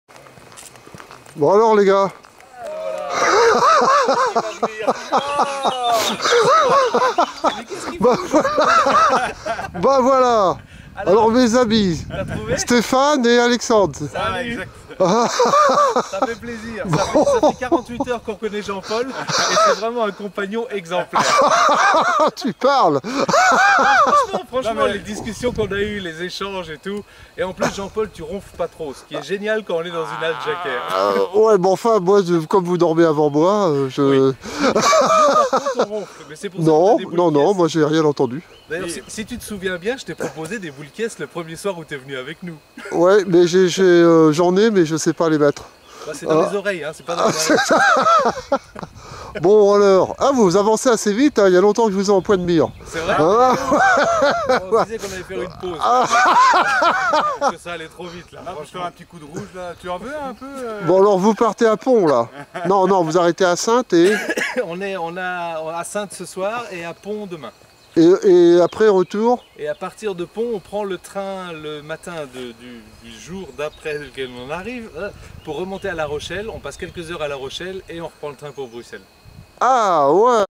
Chemin de Compostelle Fisterra 2010 de Jean-Paul Rivasseau : Stéphane et Alexandre
Nous sommes le samedi 22 mai 2010. J'ai  rencontré Alexandre et Stéphane au refuge de l'abbaye de Saint-Jean-d'Angély en  Charente maritime. Je me dirige vers Saintes, la prochaine étape. Environ 630 km ont été parcourus depuis mon départ de Roye.